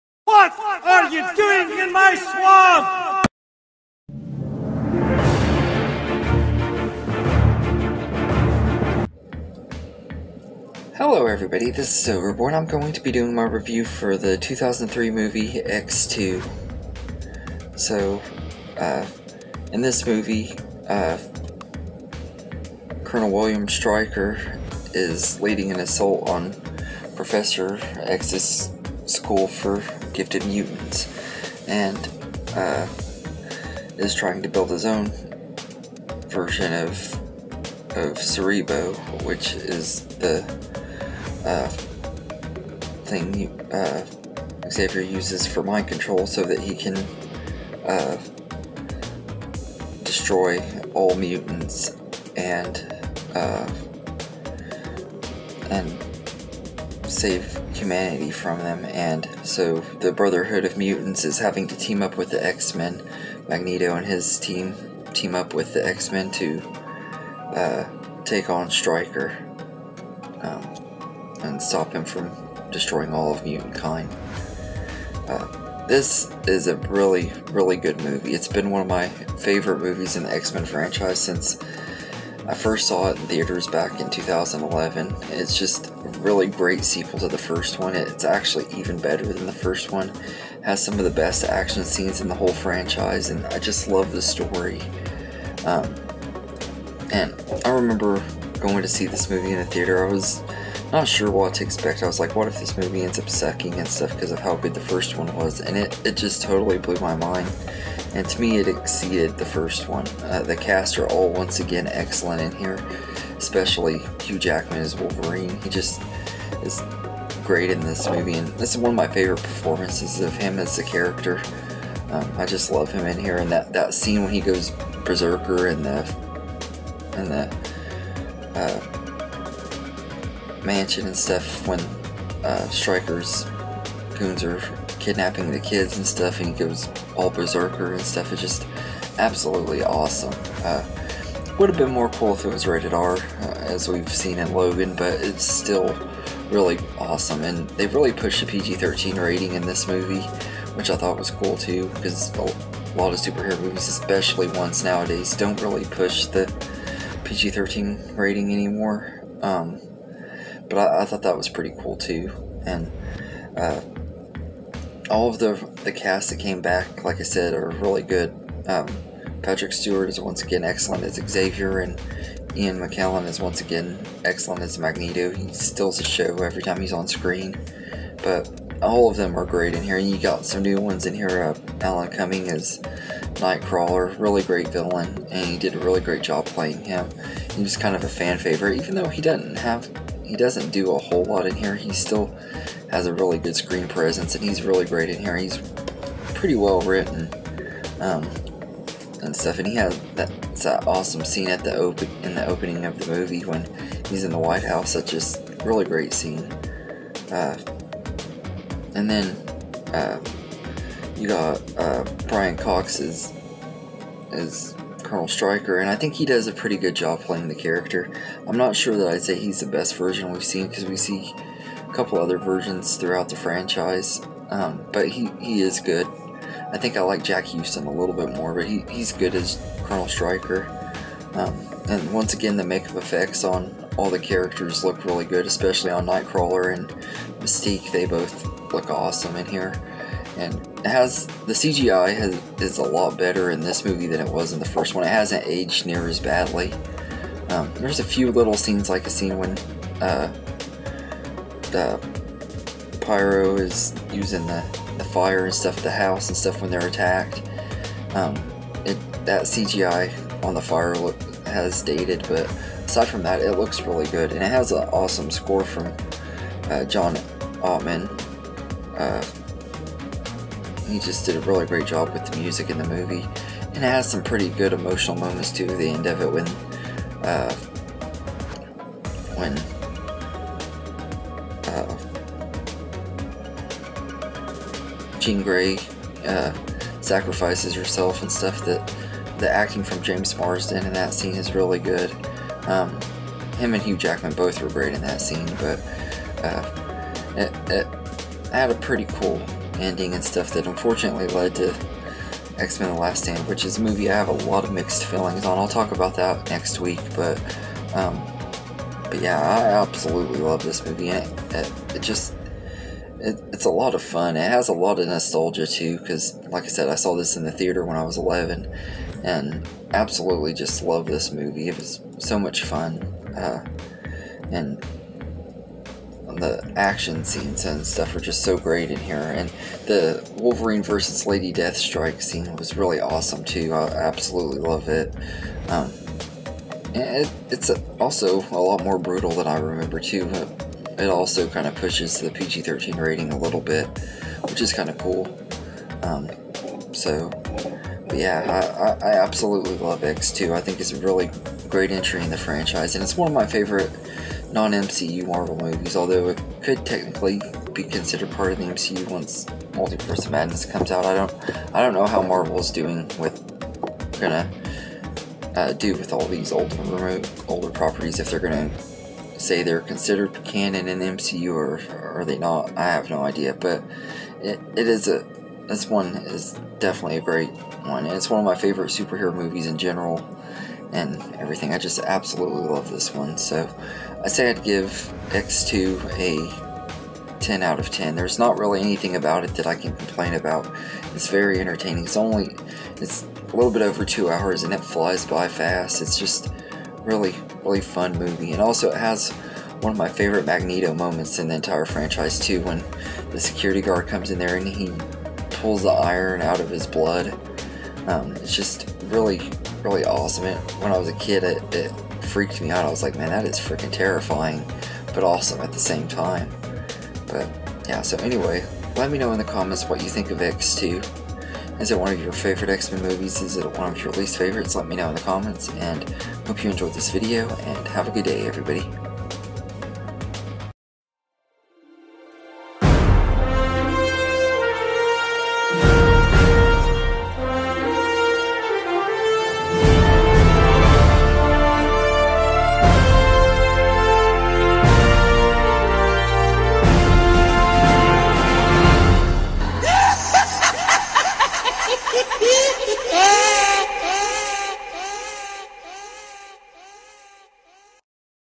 X 2 review
This week for Marvel Monday I review the second movie in the X-Men franchise.